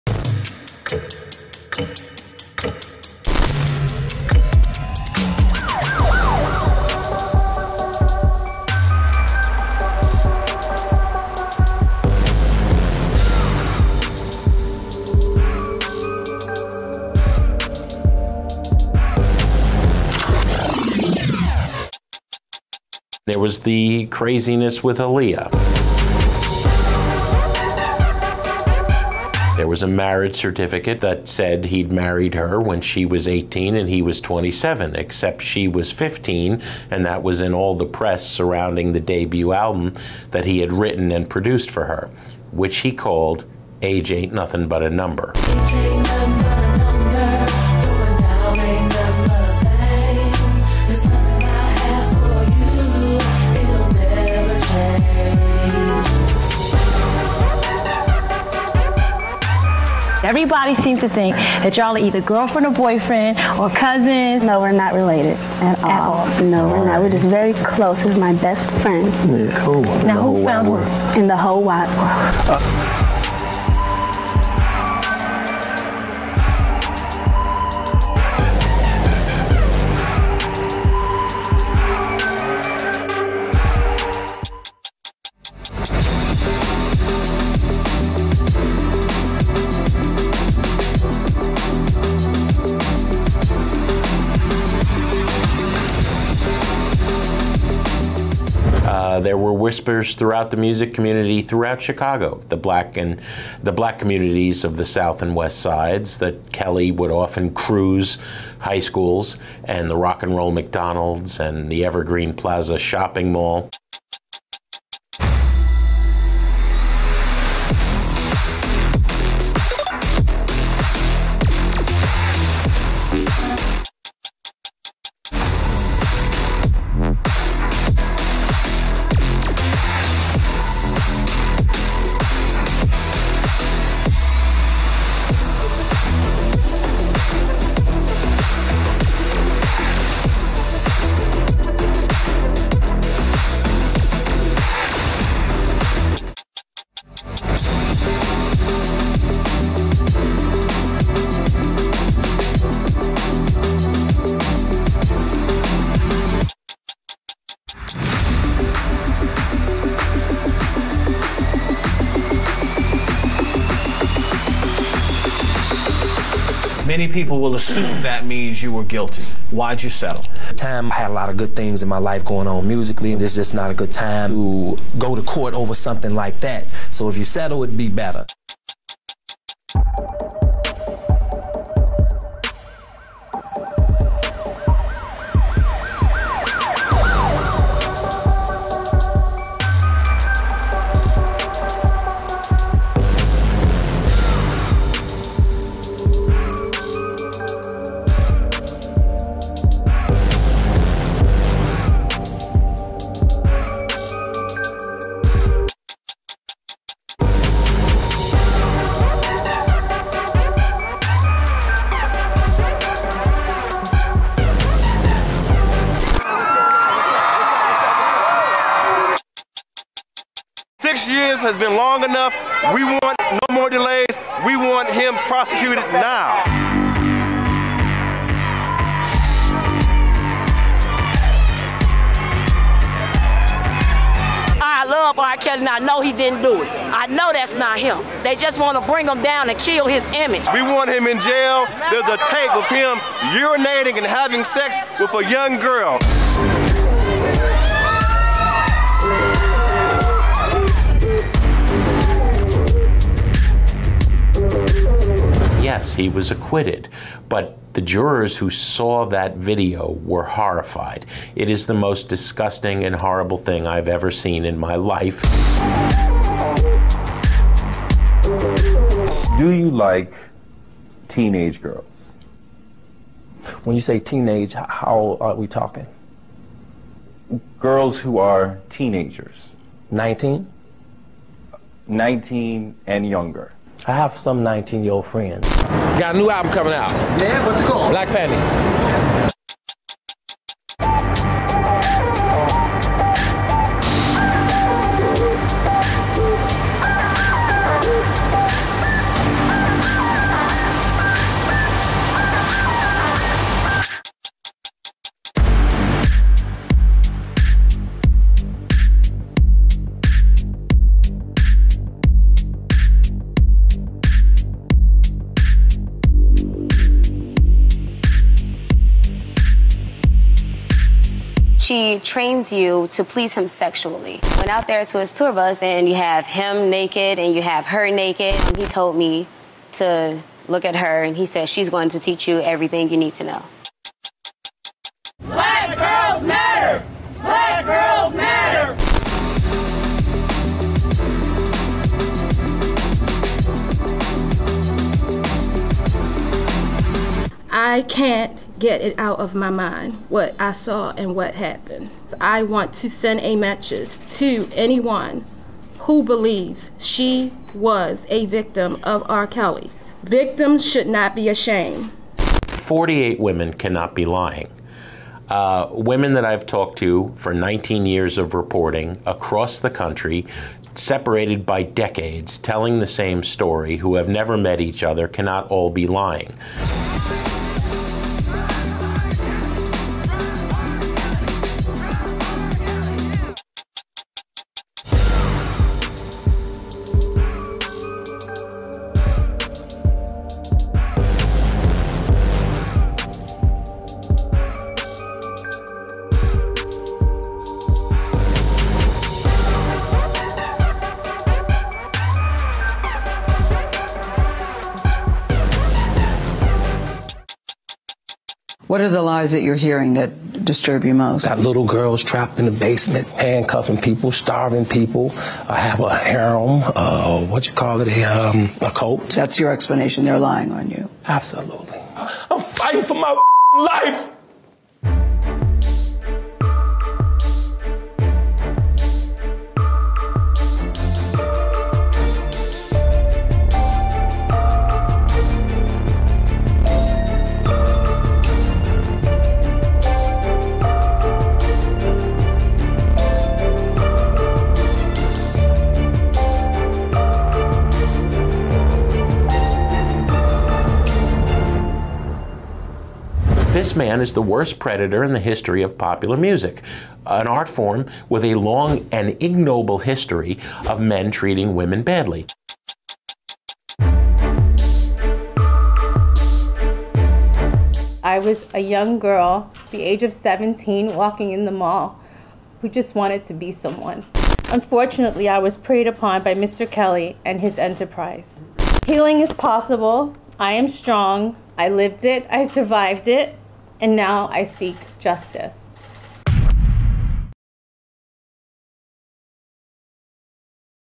R. Kelly's Sexual Misconduct Allegations: A Timeline
R. Kelly’s criminal trial has begun. Here’s a timeline of the sexual misconduct accusations he has faced over the past 30 years.